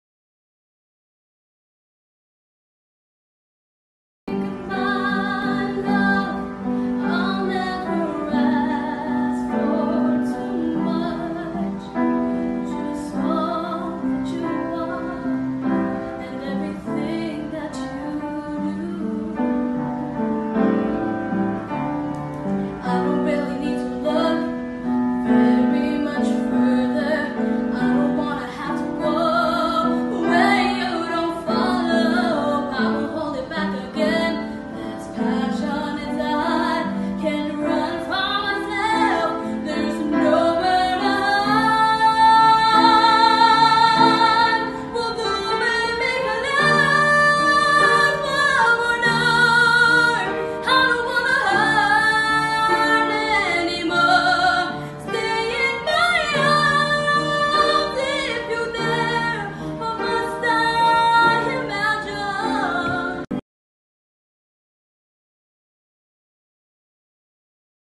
Stars at Home Brithny G. Singing
Make-A-Wish Connecticut presents Stars At Home: A Virtual Talent Show! Members of our community have submitted videos to compete in our first-ever virtual talent show. Vote for your favorite acts with a $5 donation, a virtual "high-five", and help them compete for a top spot! Give today and help us grant wishes tomorrow.